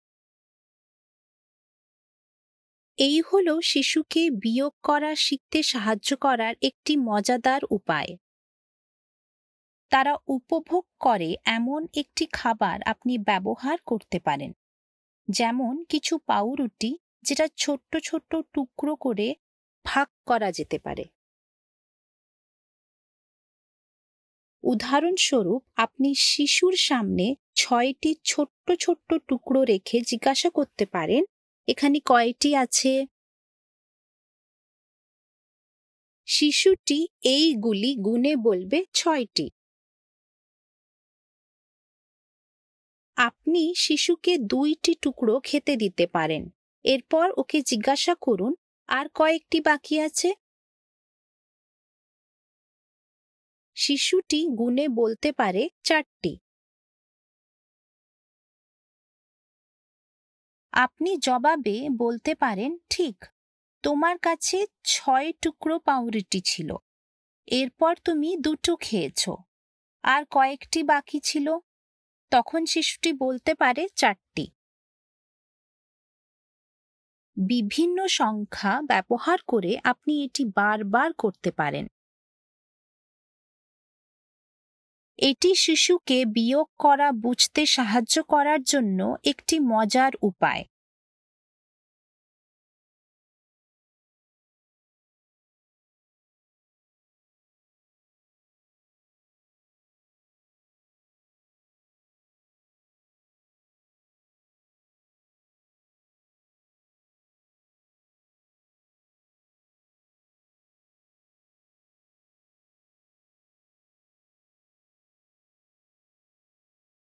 খাবার দিয়ে বিয়োগ করা - Subtracting with Food (Bengali)
খাবারের টুকরোগুলি গণনা করে, তাদের কয়েকটি টুকরি খেতে দিয়ে, এরপর বাকী টুকরোগুলি আবার গণনা করার মাধ্যমে আপনি শিশুকে বিয়োগ করা শিখতে সহায়তা করতে পারেন।

টিচ ফর লাইফ আমাদের বাচ্চাদের আরও ভাল শিক্ষিত করবার জন্য় এবং বিশ্বে আশা তৈরি করবার জন্য়একটি বিশ্বব্যাপী আন্দোলন।

Bengali translation by Biddut Dey and narration by Seema Gangakhedkar.

You can help a child learn about subtracting by counting pieces of food, letting them eat some pieces, and then counting the remaining pieces again.